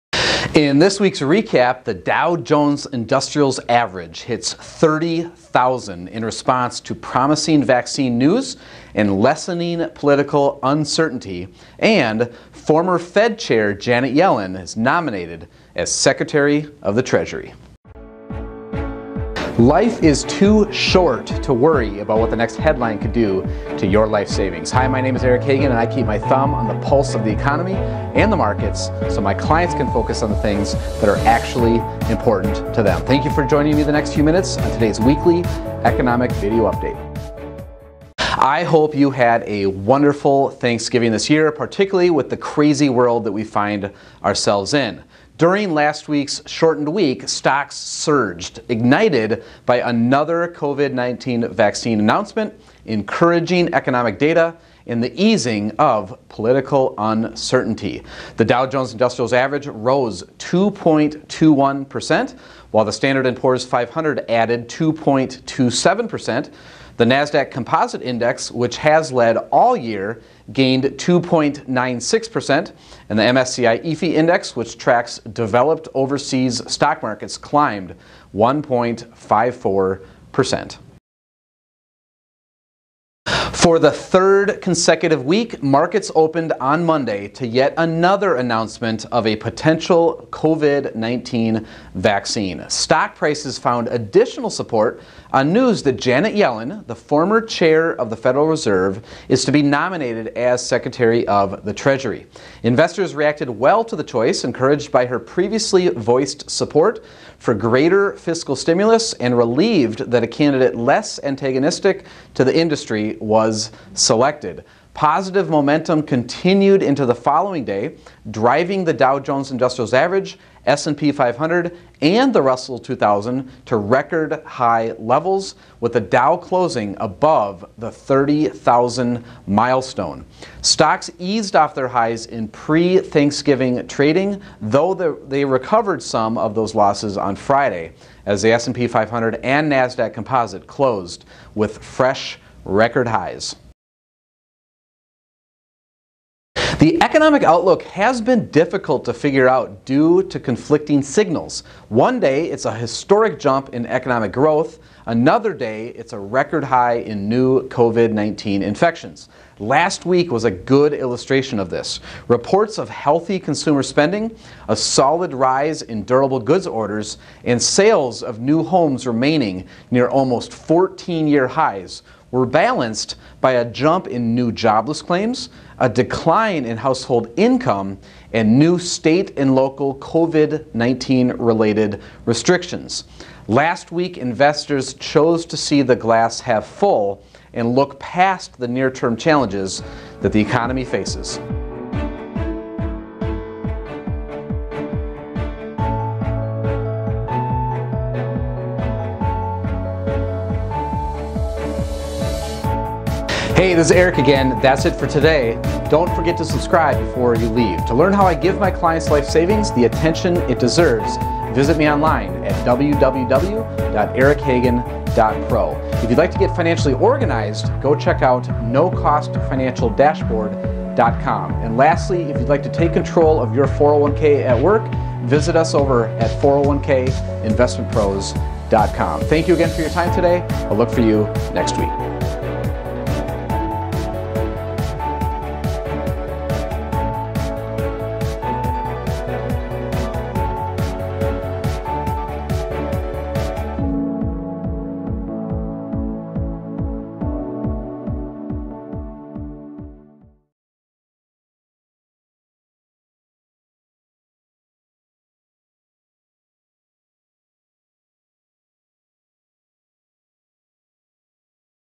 DOW HITS 30,000 MILESTONE WEVU 11/30/2020
In this week’s episode: Dow Jones Industrial Average hits 30,000 milestone in response to promising vaccine news and lessening political uncertainty; former Fed chair Janet Yellen nominated as Secretary of the Treasury.

► Did you know Eric is the father of 7? Watch his story each day to get a behind-the-scenes glimpse of his busy life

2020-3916